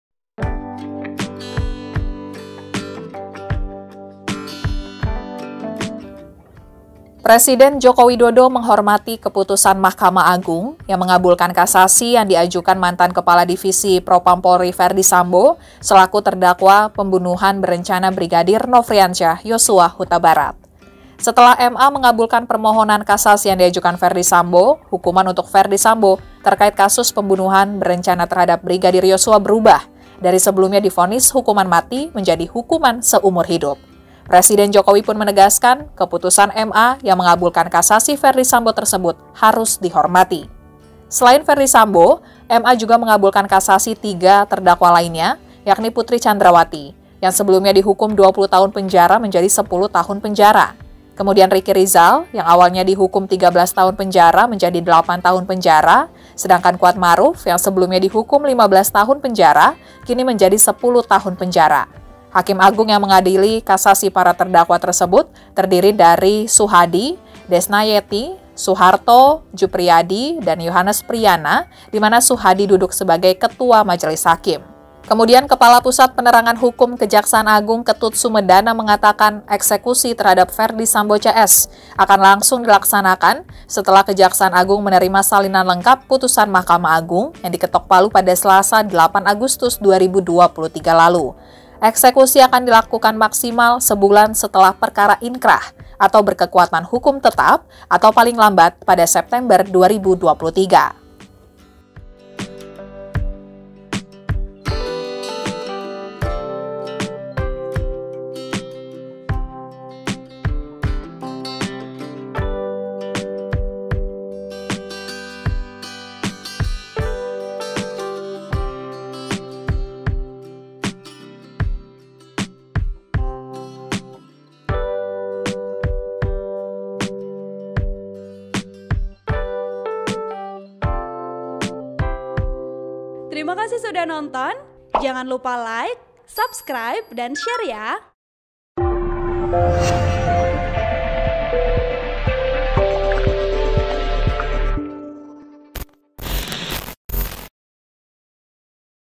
Jokowi Hormati Putusan Kasasi MA Ferddy Sambo yang Diskon Hukumannya Jadi Seumur Hidup
TRIBUN-VIDEO.COM - Presiden Joko Widodo (Jokowi) mengatakan menghormati keputusan Mahkamah Agung (MA) yang mengabulkan kasasi yang diajukan mantan Kepala Divisi Propam Polri Ferdy Sambo. Akibat putusan MA tersebut Sambo batal dihukum mati.

"Saya menghormati keputusan yang ada," kata Jokowi usai menjajal LRT di Stasiun Dukuh Atas, Jakarta, Kamis (10/8/2023).

Presiden kembali menegaskan keputusan MA yang mengabulkan kasasi tersebut haruslah dihormati.